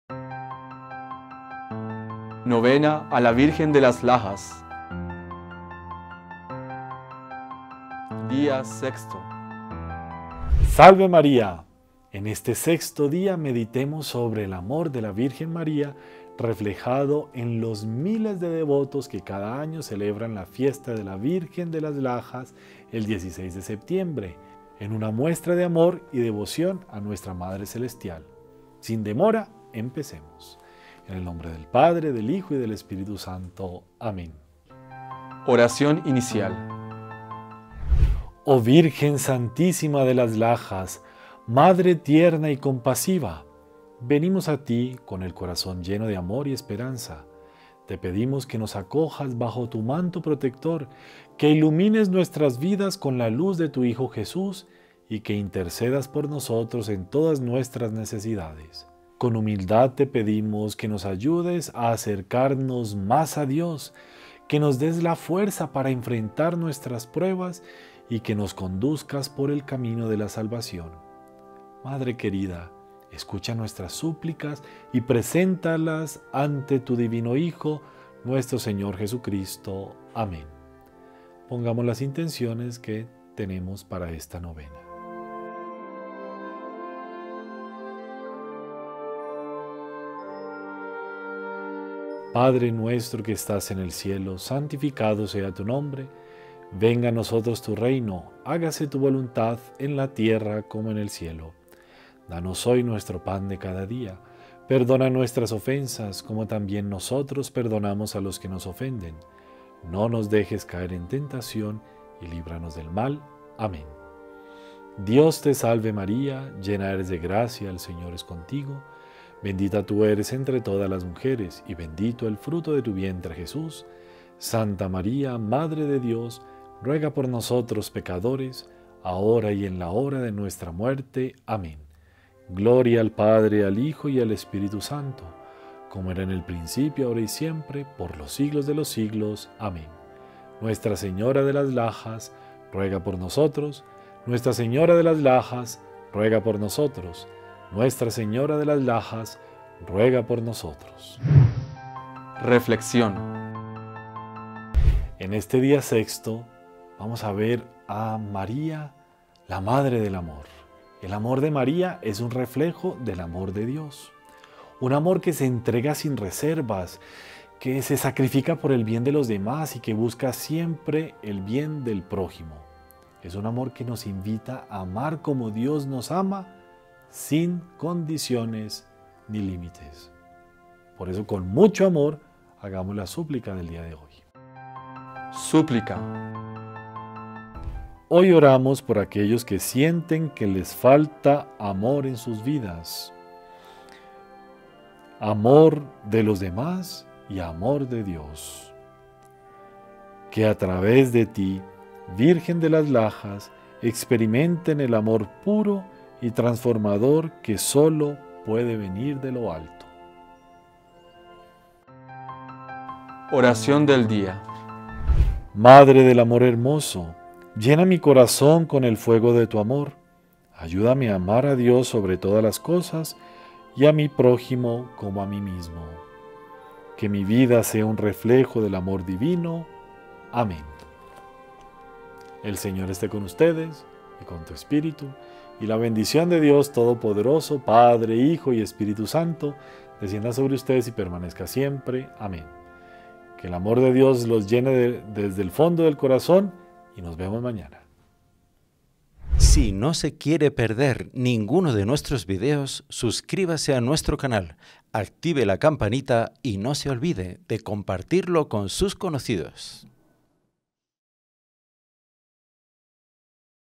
Novena a Nuestra Señora de las Lajas Día 6 con el Padre José Bernardo
👈 PDF de Nuestra Señora de las Lajas, Entra aquí ya mismo. En el sexto día de nuestra novena a Nuestra Señora de las Lajas, meditamos sobre el amor de María, un reflejo perfecto del amor de Dios.

Su amor se entrega sin reservas, se sacrifica por el bien de los demás y busca siempre el bienestar del prójimo. Este amor incondicional nos invita a amar como Dios nos ama, sin condiciones ni límites. 🙏

Acompáñanos en oración, pidiendo a María que nos ayude a vivir con la misma humildad y confianza en el amor divino que ella mostró.

¿Quieres ofrecer la Misa por tus intenciones?  Diligencia este formulario